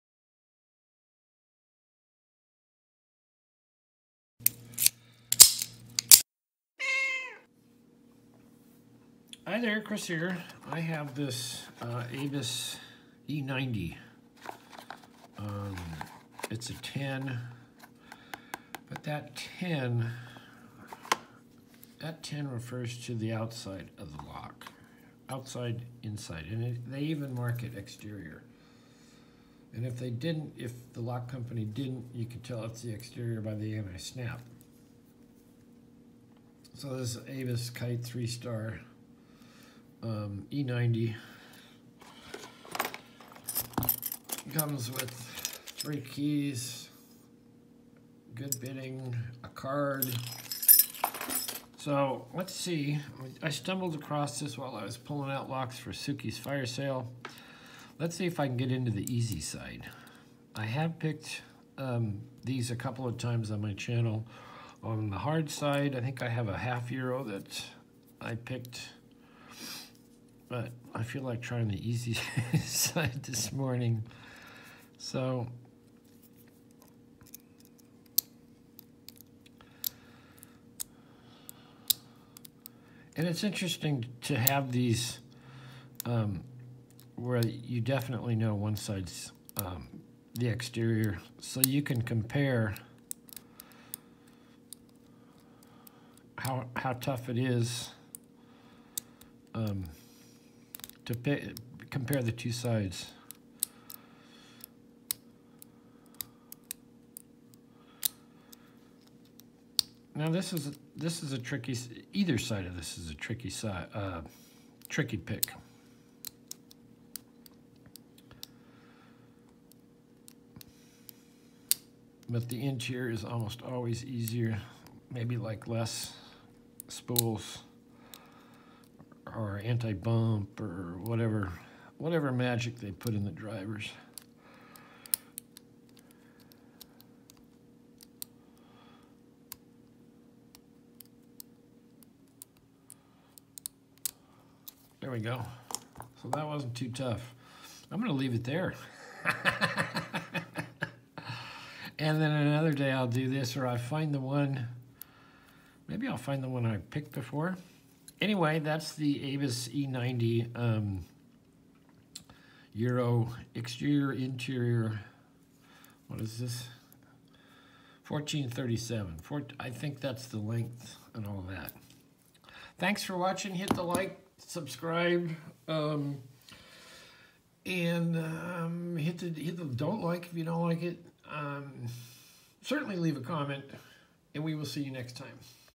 #842 Abus E90 picked interior side😅
locksport Abus E90 picked interior side, a bit easer pick😀